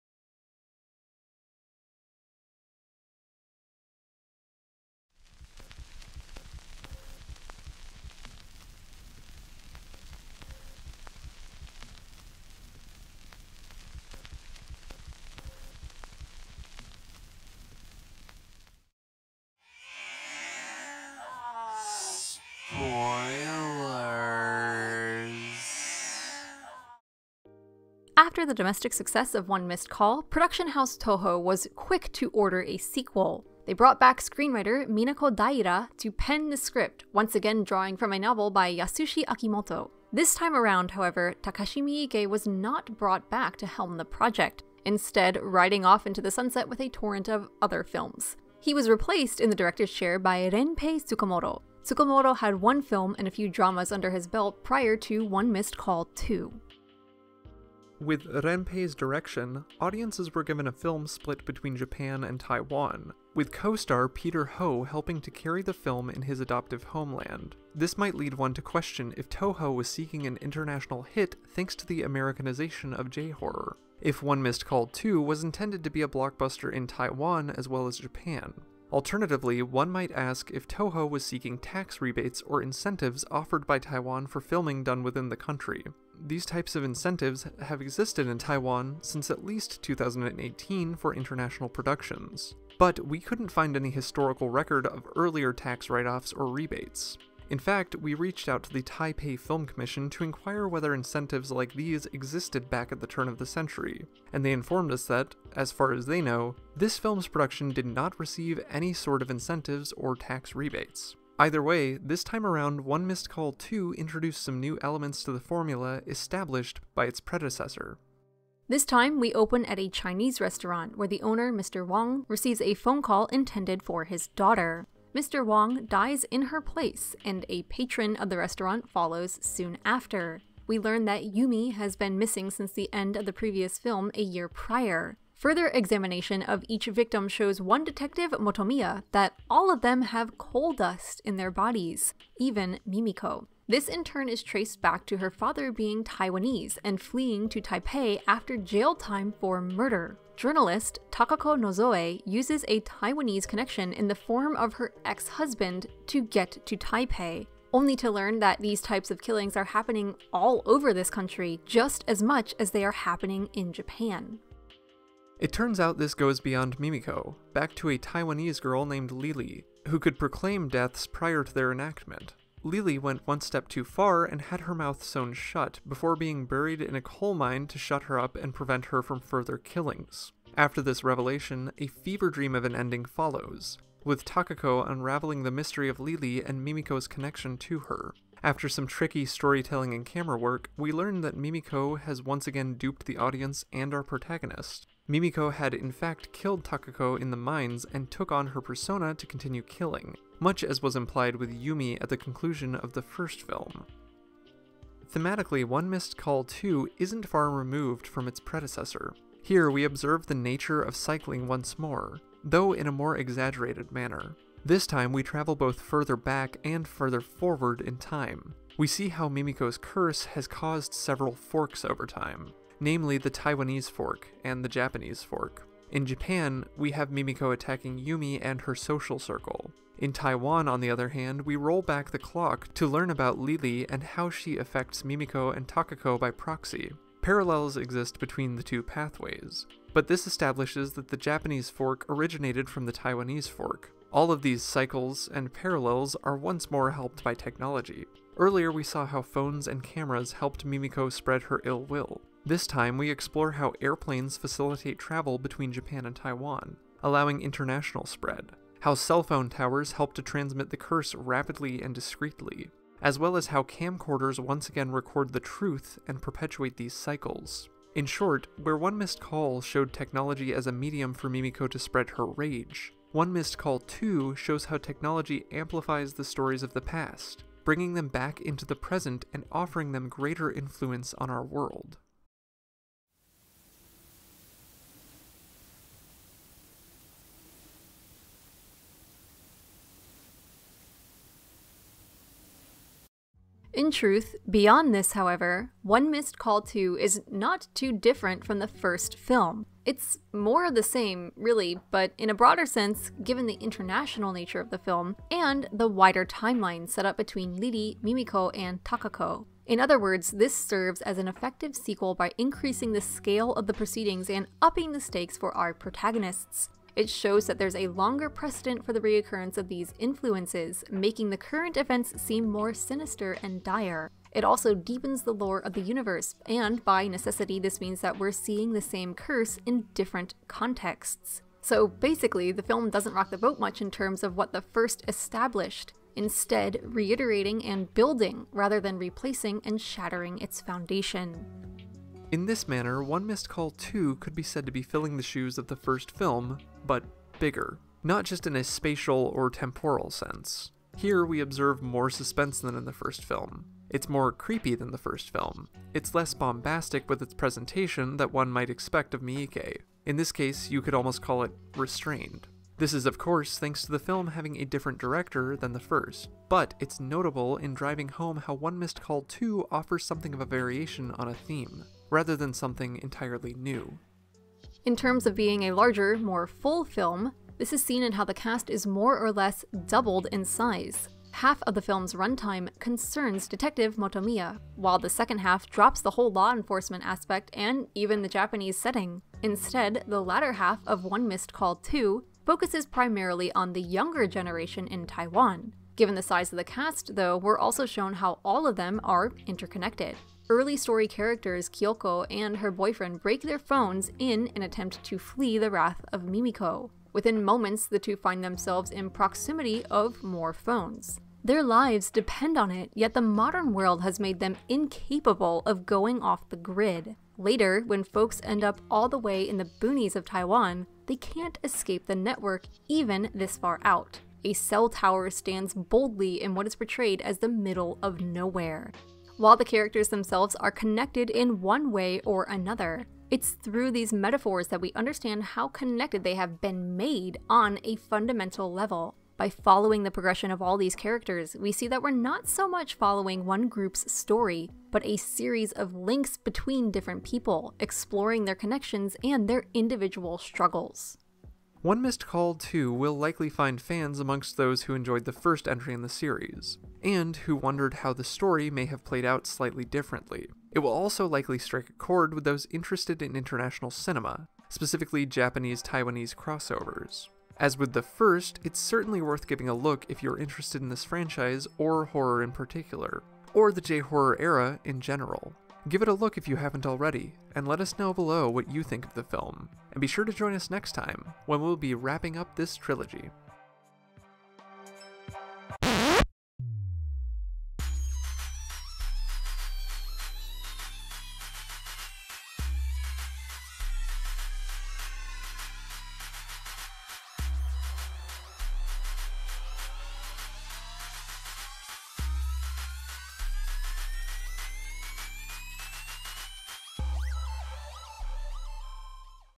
The International Relations of One Missed Call 2 | Cinema Nippon
This time around, One Missed Call goes overseas.

Review copy provided by Arrow Video.

Sound Design:
Pixabay / FreeSound
Theme song by Abysmii